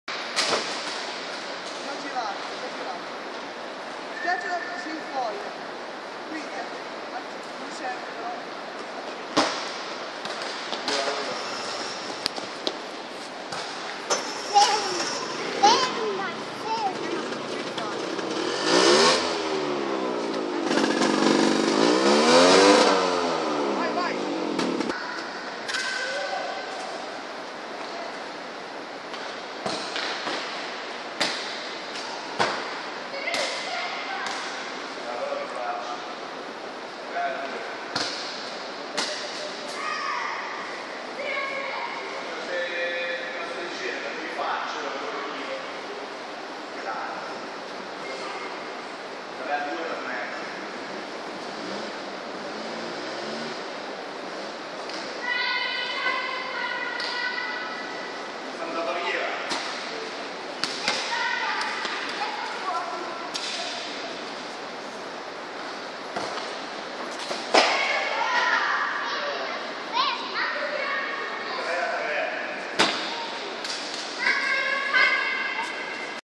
Giovane handler si allena con un pastore dell'asia centrale
Leo e Alena (Tigrova Muhari) femmina di 7 anni ... condotta con distrazioni!